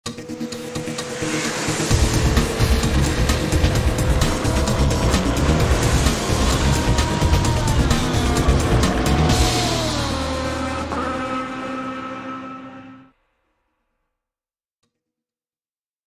Create a Speedometer Logo Video - MakeWebVideo.com
Rev up your brand with a custom Speedometer Logo Intro Video!

Showcase your unique qualities and leave a lasting impression on your target audience with this animated logo video. Choose our monthly subscription plan to access all templates and features without affecting your budget. Click now to take your branding to the next level!